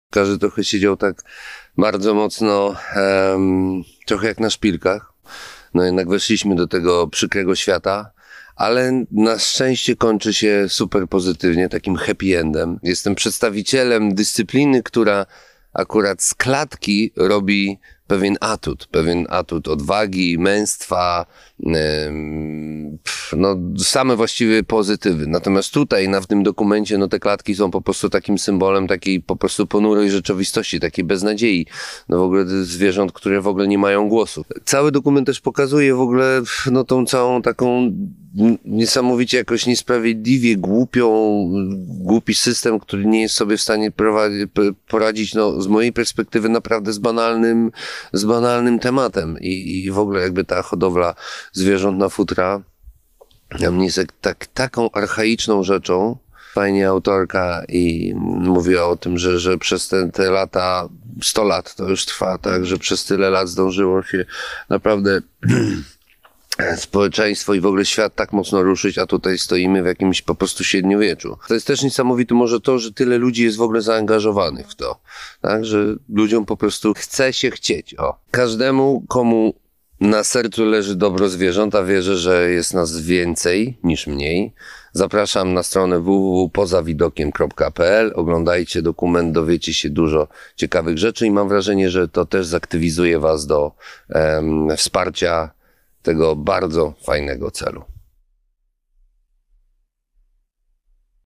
Kilka słów o „Poza widokiem, na skraju lasu" od Martina Lewandowskiego, współzałożyciela KSW
W świecie Martina Lewandowskiego klatka jest atutem męstwa i odwagi. 🥊 W filmie „Poza widokiem, na skraju lasu” jest zupełnie inaczej.
Jakie wrażenie zrobił na współzałożycielu polskiej federacji KSW dokument ujawniający kulisy walki o zakaz hodowli zwierząt na futro?